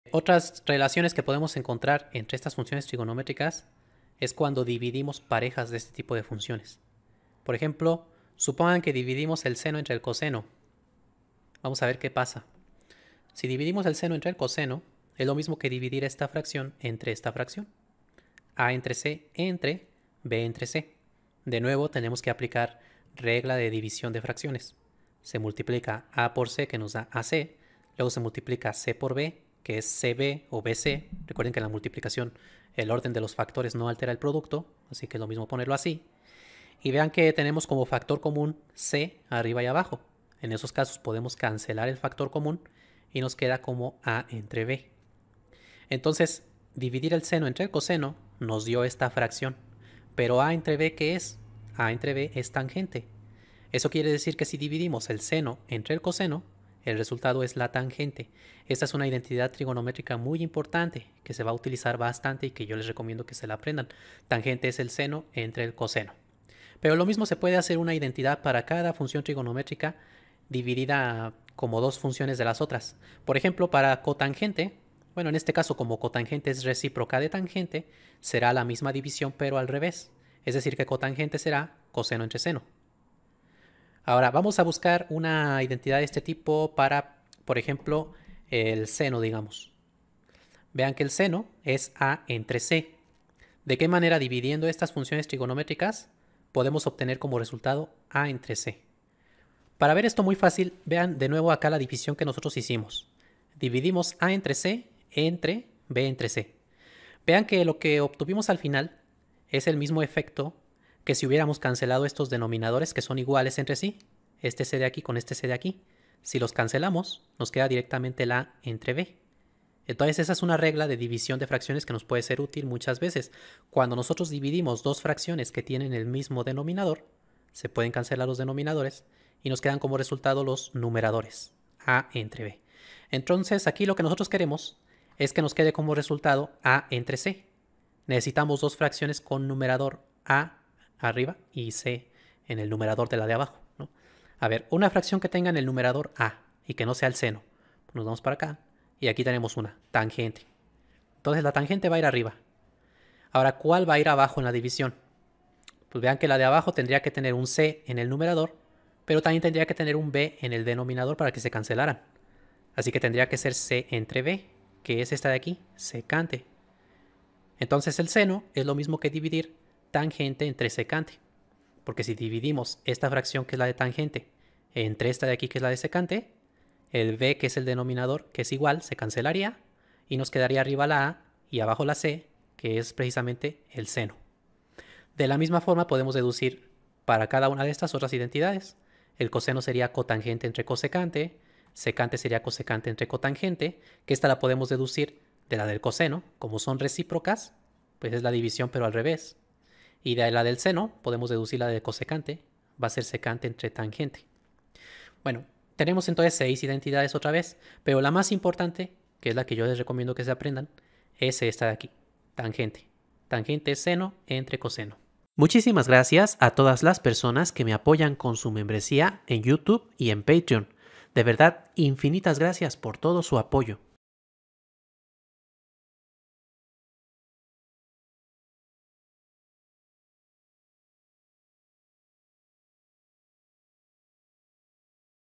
Demostración de identidades trigonométricas
En este video mostraré de qué manera se deducen las identidades que relacionan a las funciones trigonométricas mediante cocientes, a partir de las definiciones en un triángulo rectángulo, paso a paso.

** MIRA TODOS MIS CURSOS AQUÍ **
** BIBLIOGRAFÍA **
- Trigonometría, Swokowski
- Geometría y trigonometría, Baldor
- Precálculo, James Stewart
- Matemáticas Simplificadas, de Conamat
** DONACIONES **